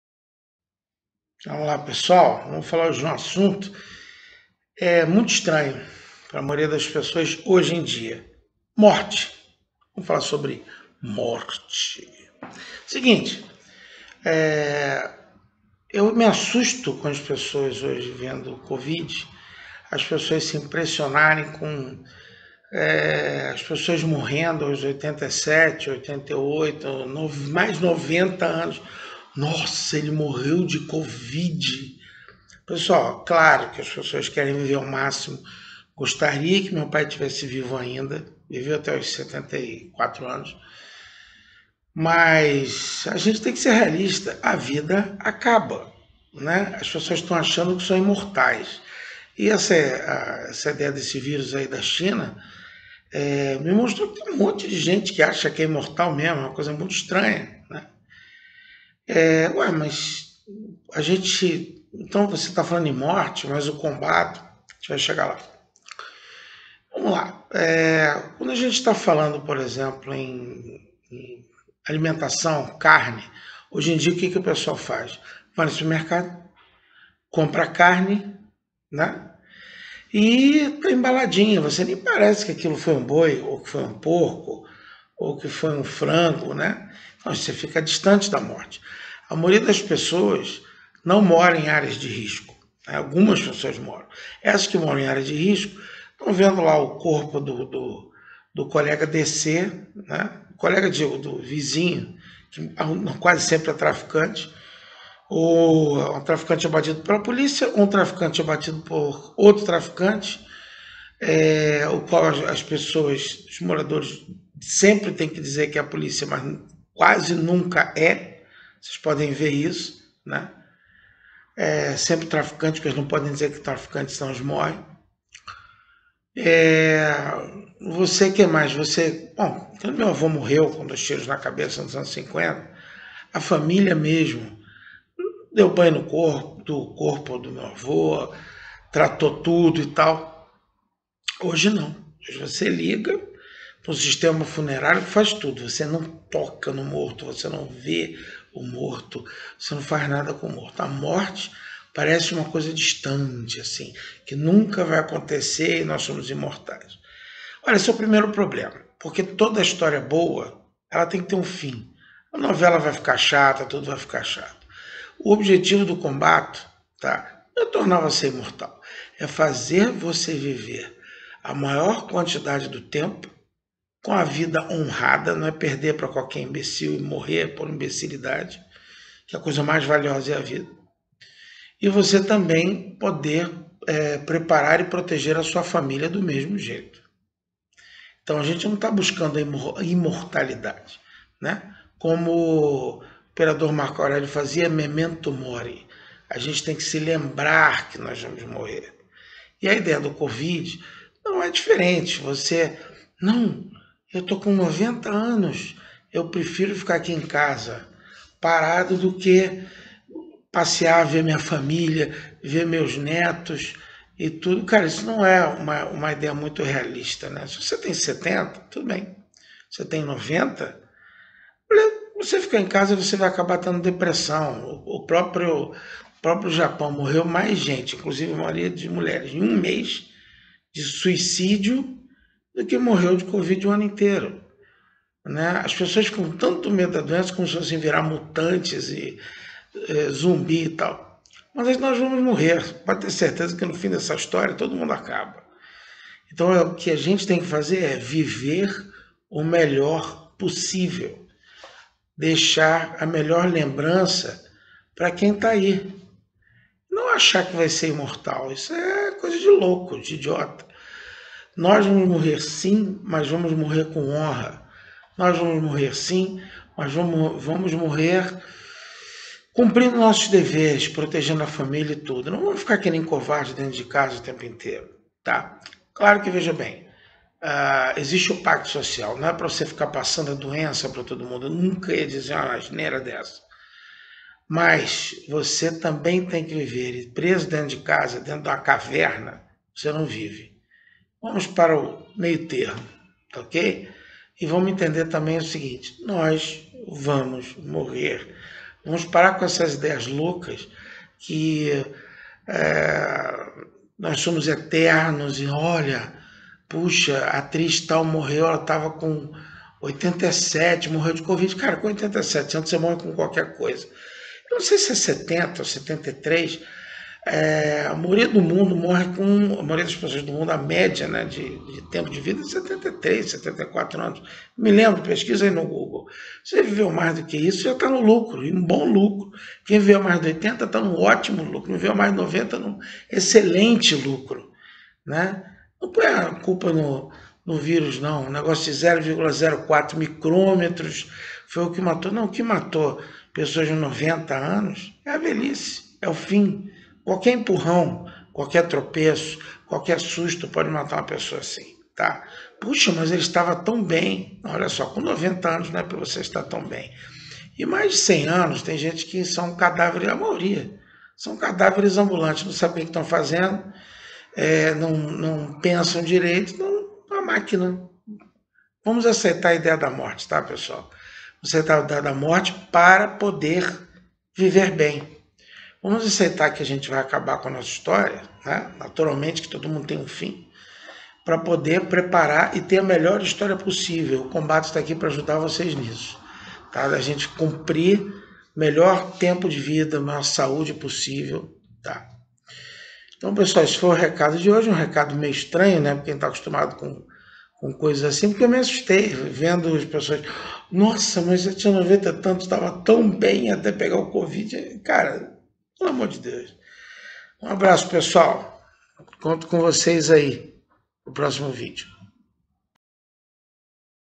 CONSCIÊNCIA DA FINITUDE!
Mestre Paulo Albuquerque explica porque as pessoas estão perdendo a consciência do que é morrer!

Links para adquirir o novo livro do Mestre Paulo
Livro de papel (livro físico)

Livro para Kindle: